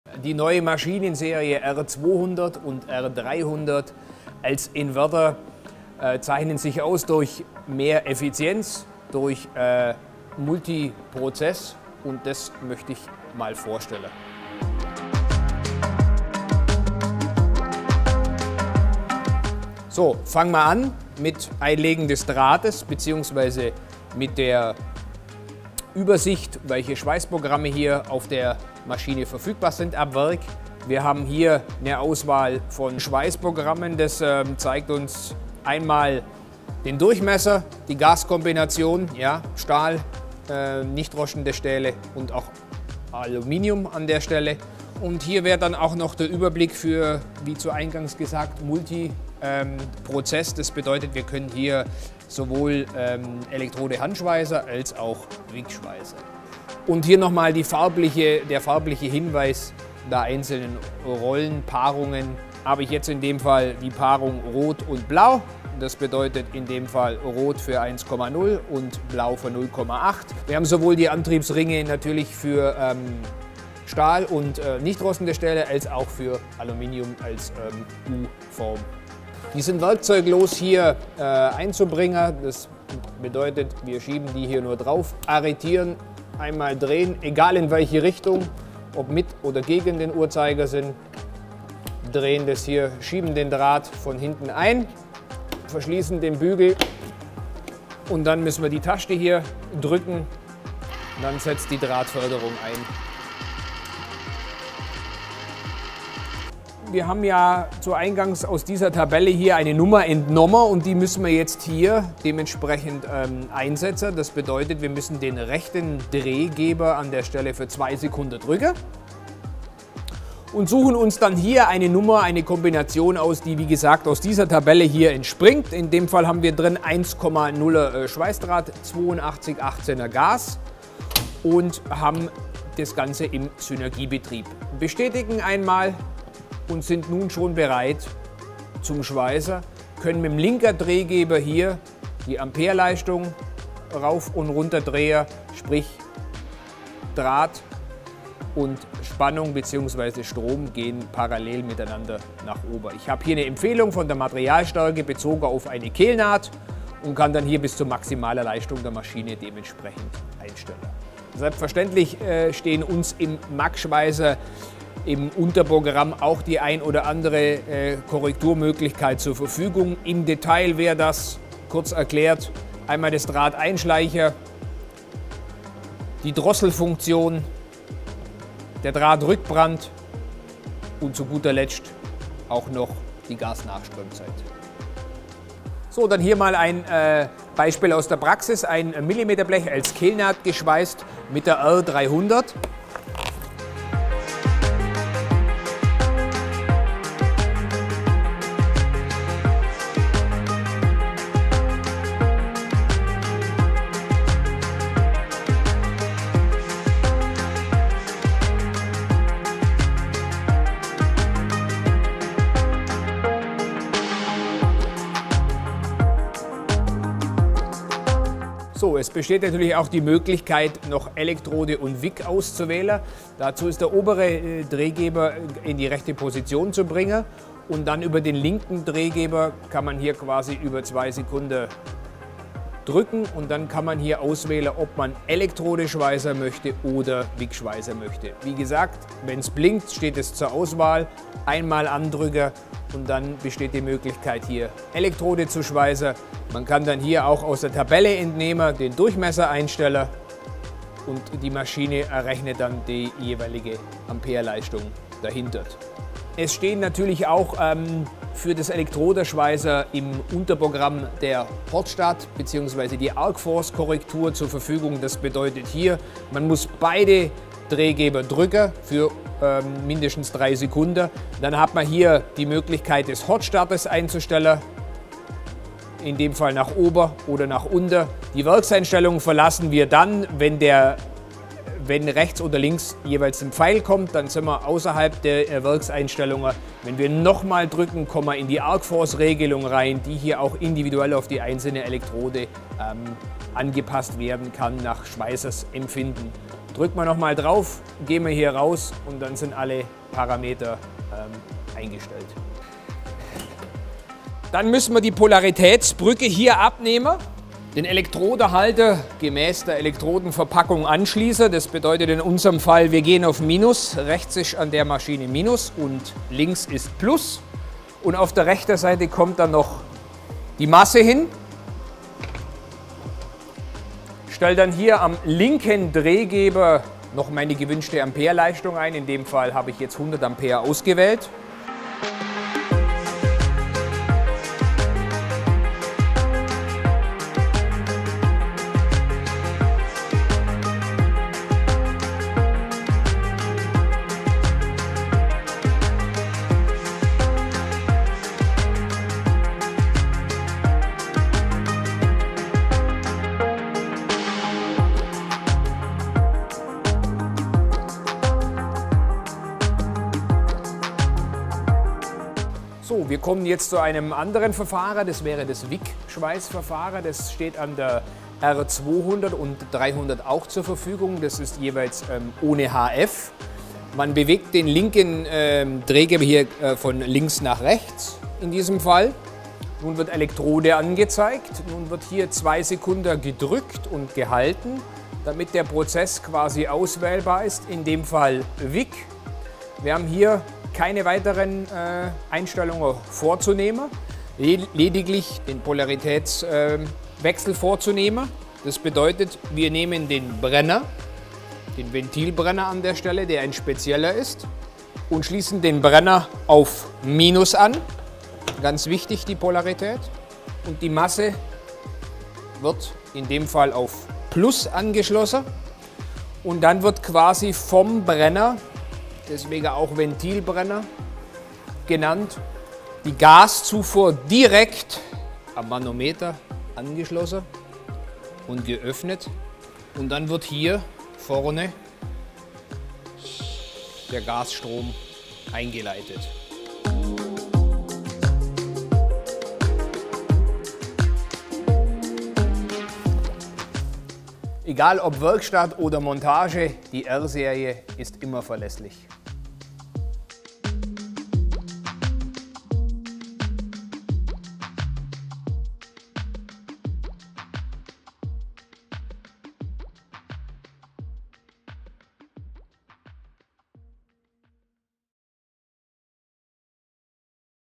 Bedienungsanleitung Schweißgeräte der Lorch R-Serie: Robust, extrem effizient und leicht zu bedienen
💡 Mehr Infos und Kaufmöglichkeit (Link zum Produkt auf der Lorch-Webseite)

In diesem Video erfahrt ihr, wie einfach und effektiv das Schweißen mit den Schweißgeräten der Lorch R-Serie ist! 🔥

Die Lorch R-Serie: robust, kompakt und perfekt für Werkstatt und Montage. Invertertechnologie für optimale Schweißnähte bei einfacher Bedienung. Synergie-Einstellautomatik für perfekte Ergebnisse ohne Einlernen.

🔧 Einfache Bedienung am intuitiven Grafikdisplay
🛡️ Maximale Sicherheit mit abgewinkeltem Brenneranschluss, doppelter Gasflaschensicherung und Wegrollsperre
🌐 Flexibilität für MIG-MAG, WIG oder Elektrode

Schaut euch das Video an und entdeckt, wie die R-Serie euren Arbeitsalltag erleichtert! 🚀

Videoinhalt:
0:00 Intro, Einlegen des Drahtes, Übersicht Schweißprozesse
1:47 Schweißprozesseinstellungen
3:05 Beispielschweißung: Millimeterblech als Kehlnaht geschweißt mit der R300
3:38 Auswahl Elektrodenhandschweißen / WIG-Schweißen
4:16 Elektrodenhandschweißen: Hot Start + Arc Force
5:02 Polaritätsbrücke abnehmen, Elektrodenhalter und Masse anschließen, Ampereleistung wählen
5:32 Beispielschweißung Elektrodenhandschweißen
6:02 WIG-Schweißverfahren ohne HF-Zündung
6:34 Polaritätswechsel, Brenneranschluss, Masseanschluss, Gaszufuhr